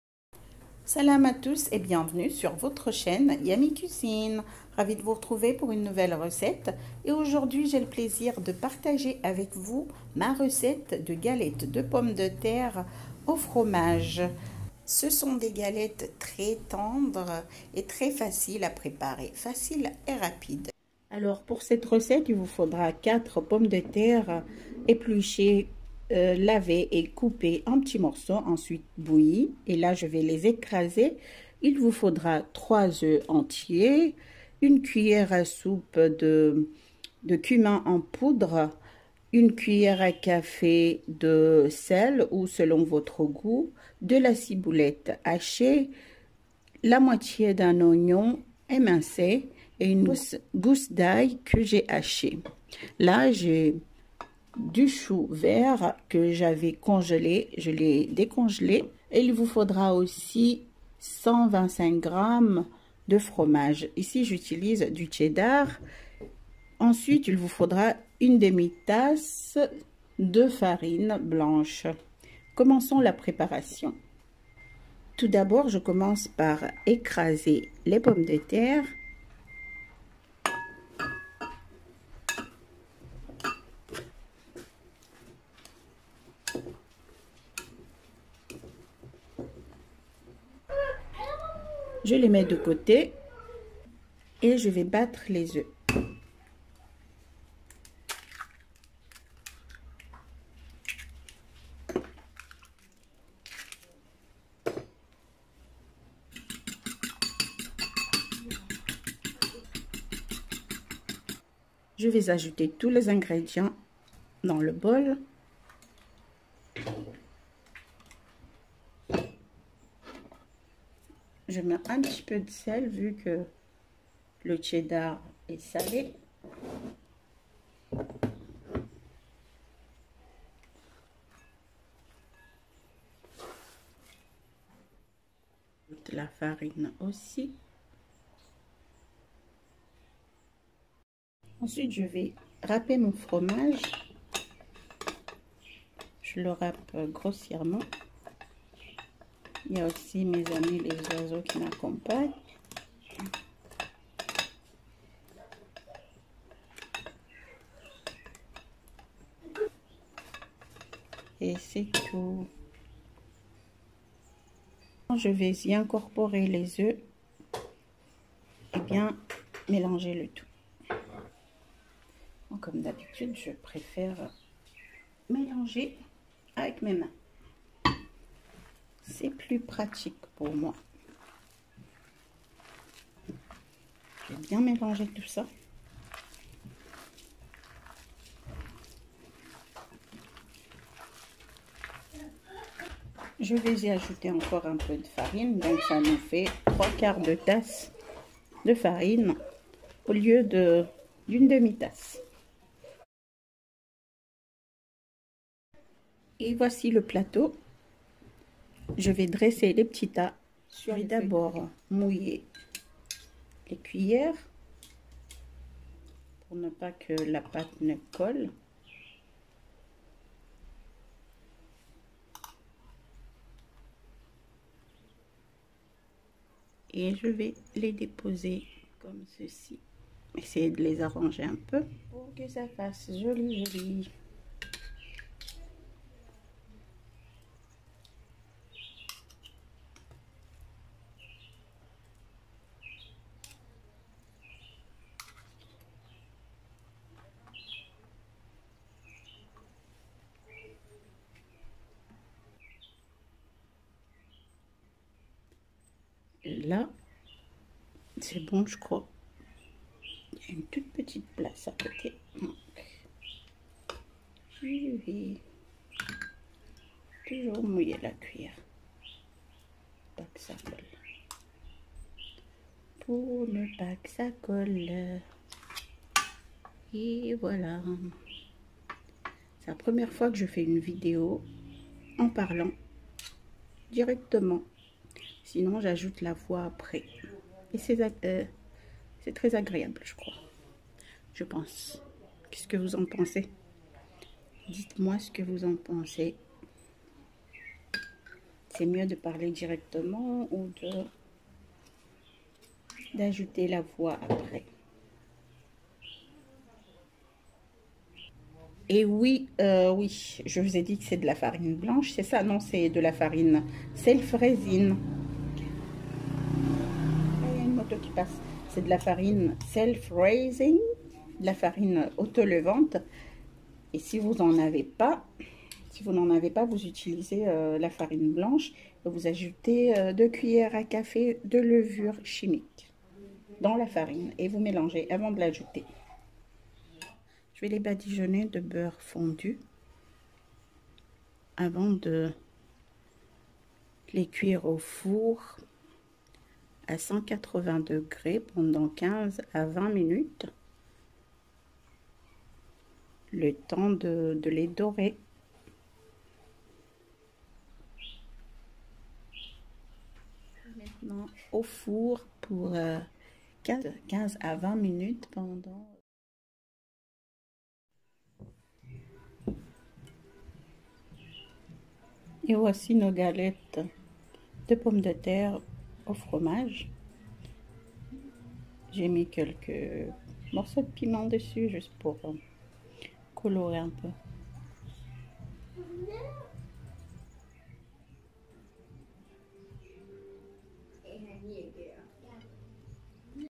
Galettes de pomme de terre au fromage, recette très facile et rapide
coucou je vous partage ma recette de galettes de pommes de terre au fromage.

ingrédients

4pommes de terre moyennes
bouillies et écrasées

125 g de fromage

1cs de cumin

1cc de del

1/2 oignon émincé

1 gousse d'ail hachée

2 cas de ciboulette hachée

3/4 tasse de farine (120g)

3oeufs

beurre fondu pour badigeonner